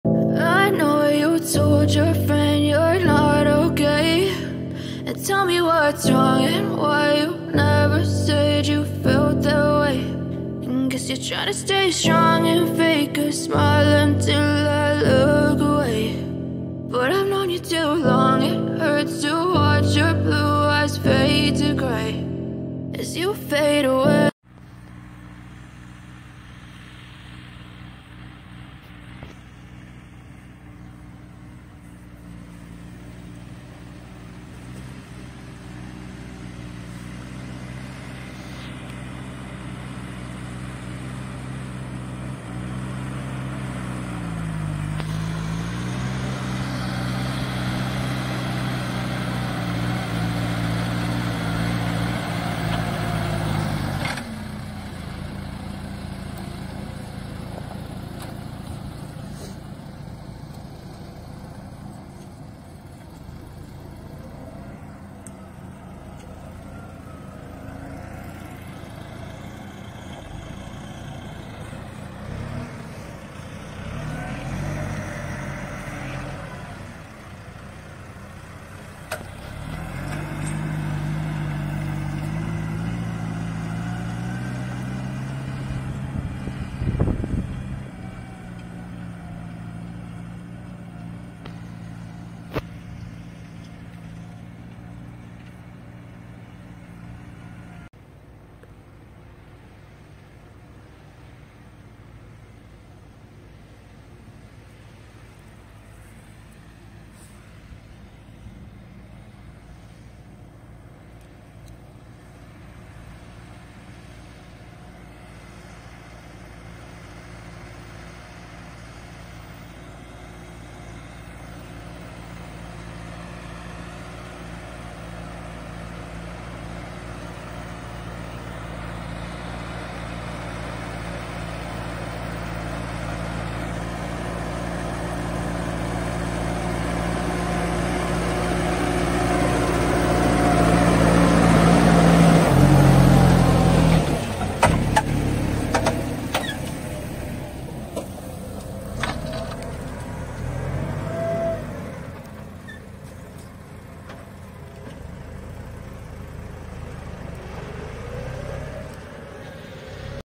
Pure sound | Mc cormick x 5 + trivomere
02/10/2022

Origgio (VA)

Ciao a tutti e benvenuti/bentornati oggi in questo video vediamo la fase di aratura dopo aver sparso il letame. L'aratro trivomere Savarin è trainato da un trattore mc.cormick x 5 dotato di caricatore frontale mc 100.
Buona visione

Si ringrazia l'azienda agricola Bonetti